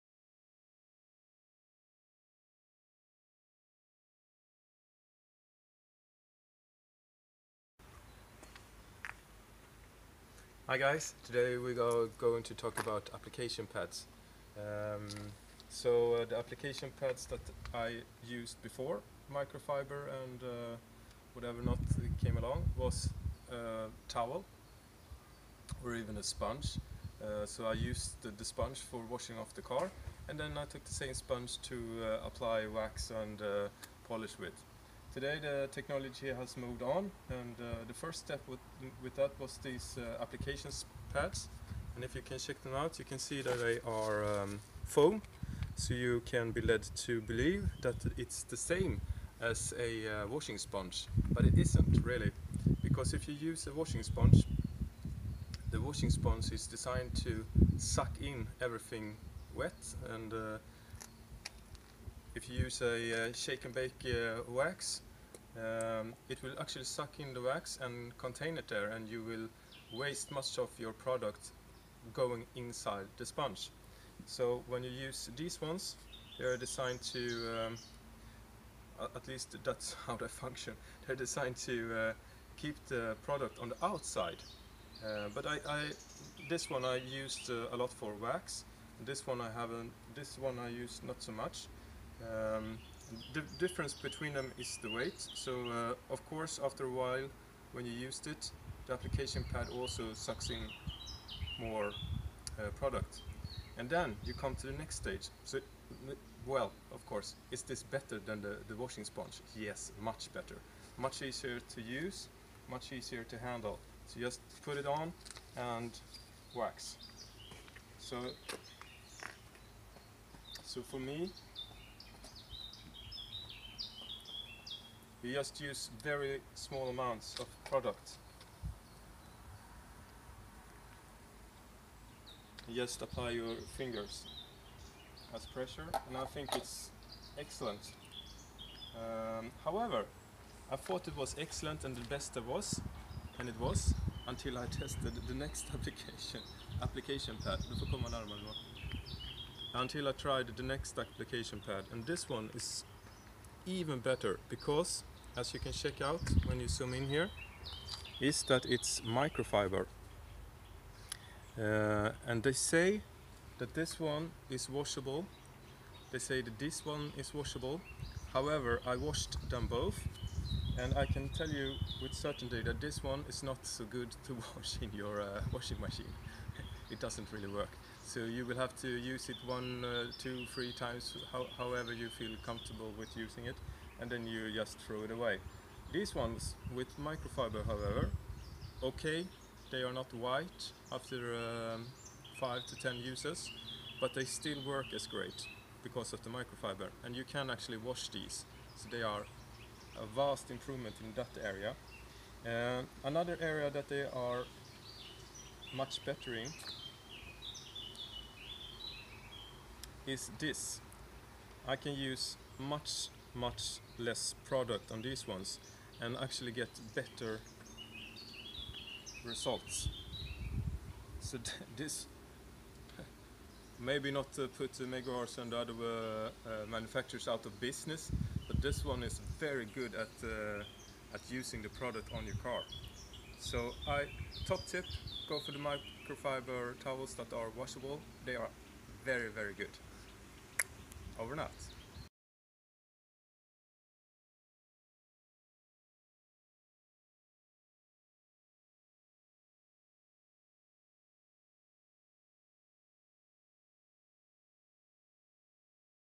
Meguiars soft foam application pad vs even coat microfibre application pad
Testing the meguairs products soft foam pad and even coat microfiber pad.
what are they like and can they be washed. are they even better than just using a sponge or towel?